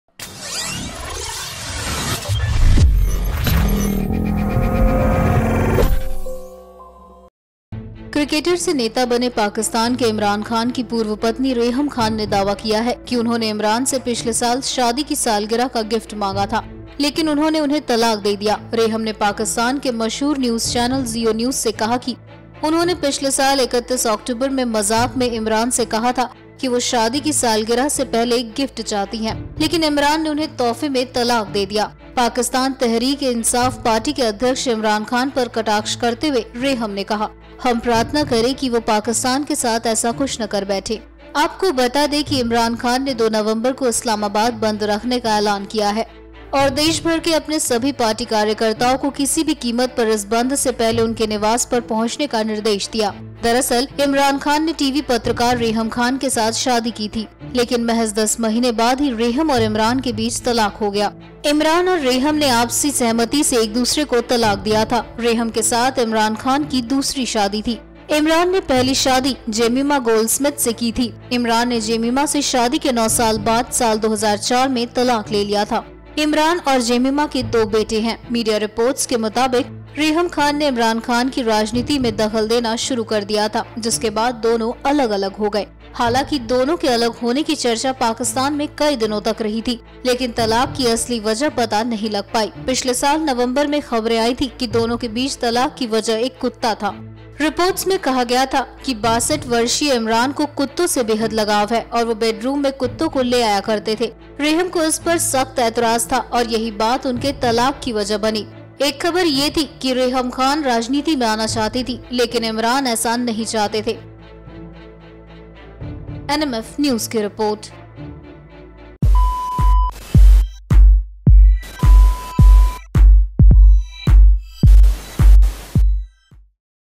Reham Khan ने किया खुलासा- Imran Khan से मांगा था anniversary gift, मिला Divorce
Imran Khan's ex-wife Reham has claimed that she had asked the cricketer-turned-politician for a wedding anniversary gift last year but he divorced her instead. Reham, while speaking to Geo news, said she had jokingly mentioned on October 31 last year that she wanted a present ahead of the couple's wedding anniversary. "He divorced me instead," she said. Taking a swipe at the Pakistan Tahreek-i-Insaf Chairman, Reham said, "Let's pray he doesn't do something like this with Pakistan."